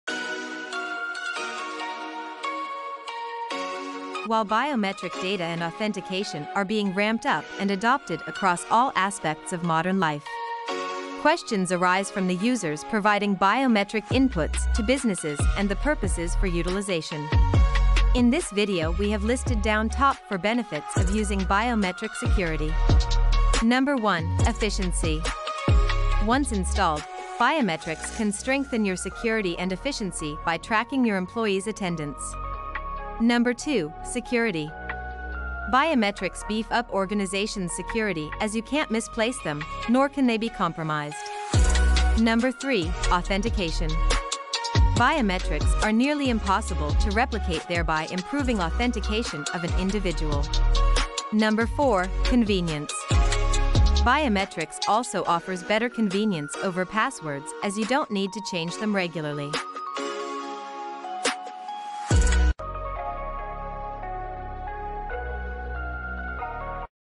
Advantages of Biometric Security | Spectra Technovision
Welcome to the future of security.

Biometrics is the most sophisticated security technology on the market, with technology that can recognize you and adapt quickly to your changing face and body.

Upgrade to biometric security, so you never have to worry about a lost key or password again.⁣⁣